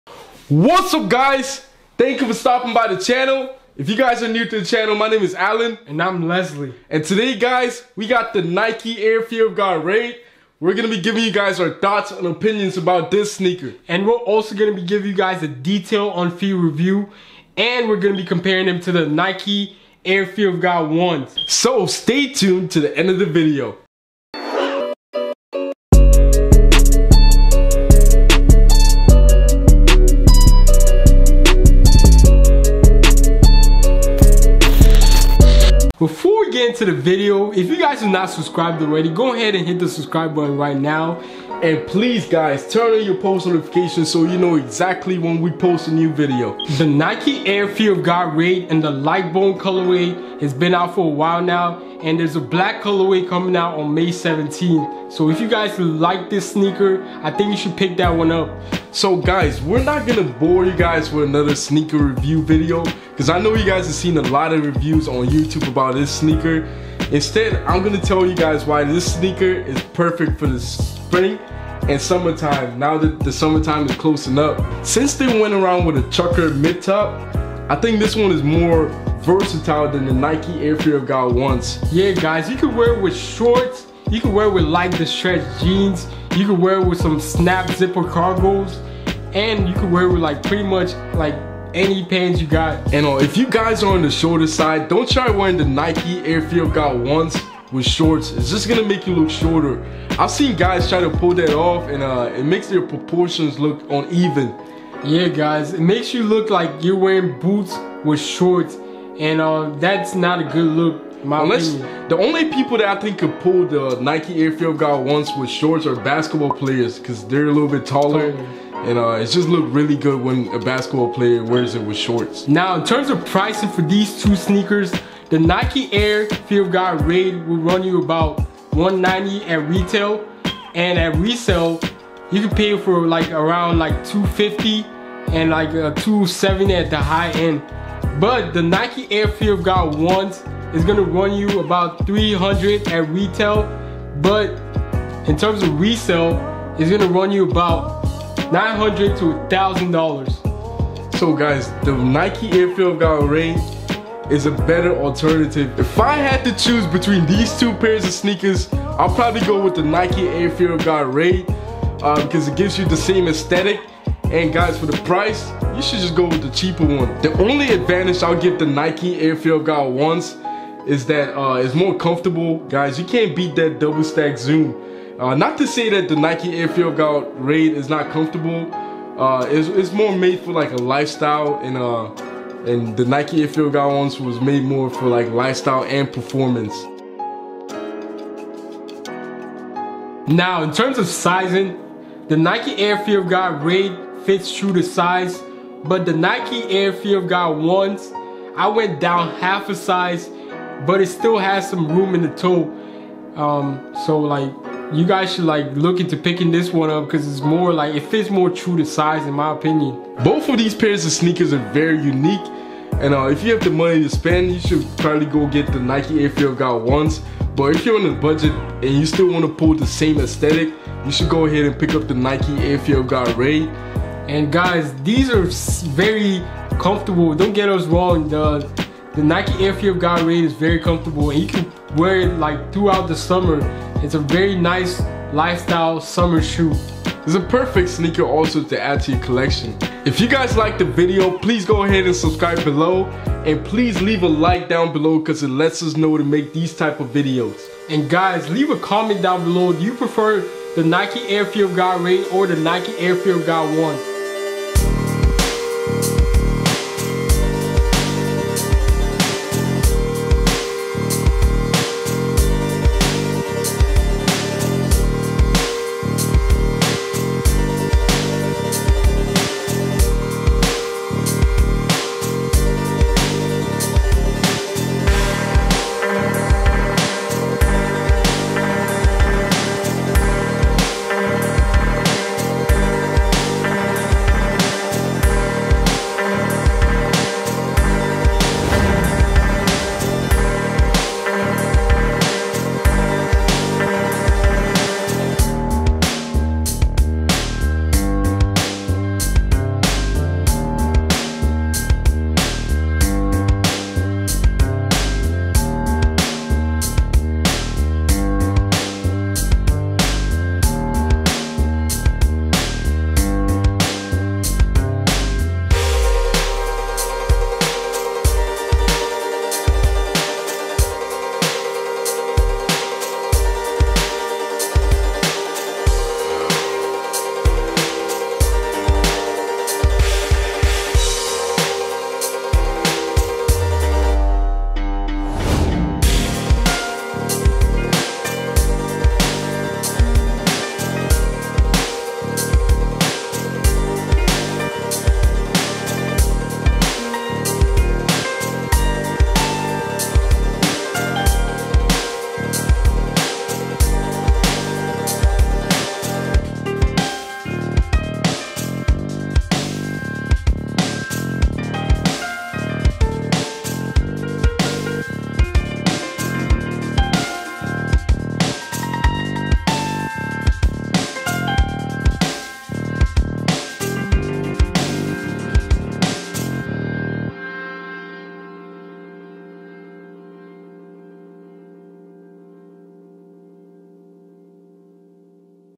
Nike air fear of god raid : Is it More Wearable than Air fear of god 1?
The nike fear of god raid  is one the newest sneaker released by jerry lorenzo and nike. It was released with the nike air fear god moc The air fear of god raid are inspired by a classic nike sneaker called the nike air raid  which jerry lorenzo was a big fan of when he was younger.